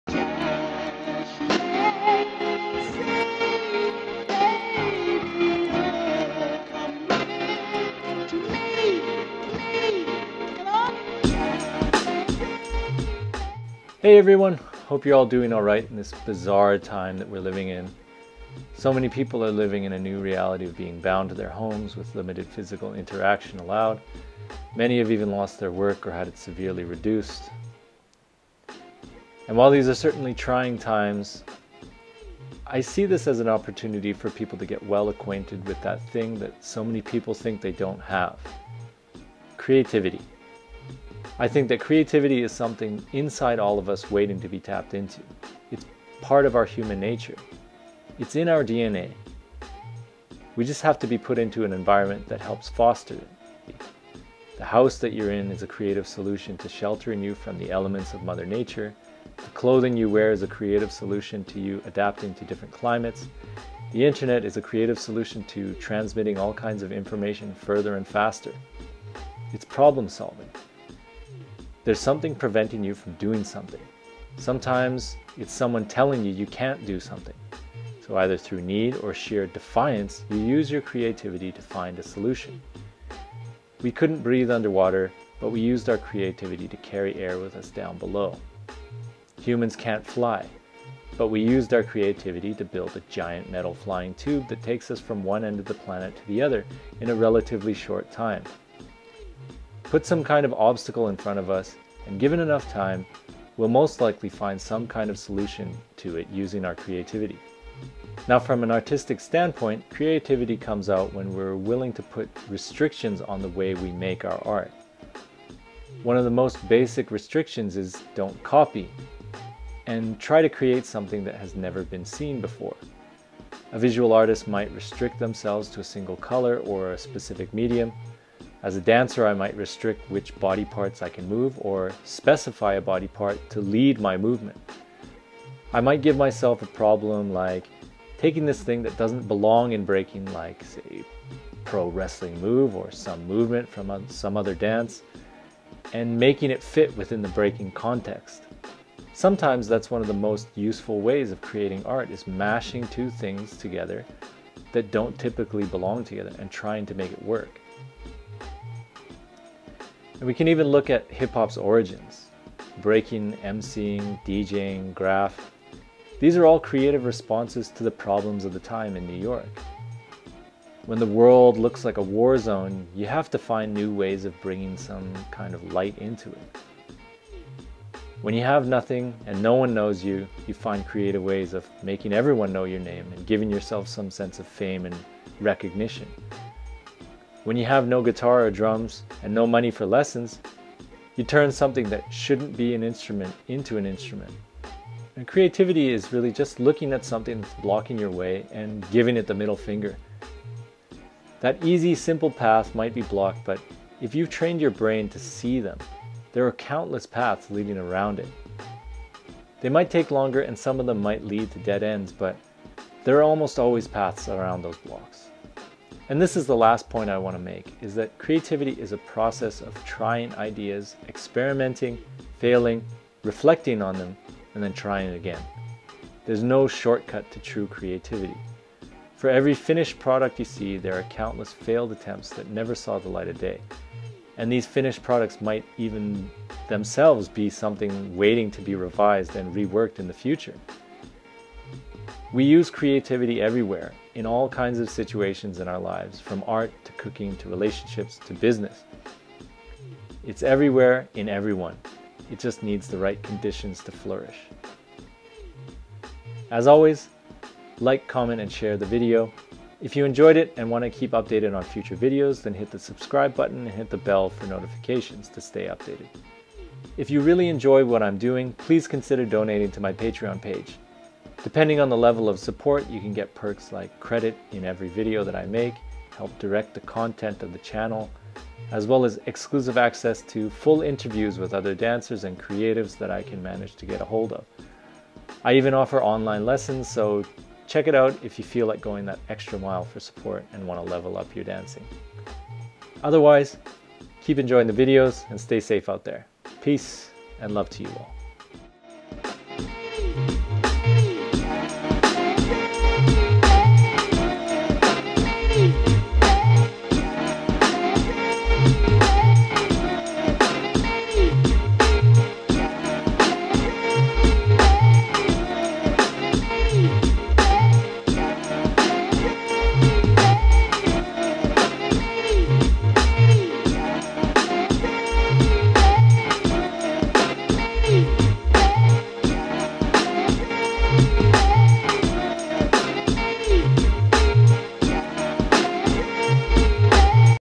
Creativity is Problem Solving - Breaking Mentality - Razzy | FILTHEE FEET
Some thoughts on the nature of creativity and how to go about thinking about it.  Put simply, there are things that block us, and we have to find solutions.  In an art context these are self imposed and that's part of the fun is finding new puzzles for you to solve yourself.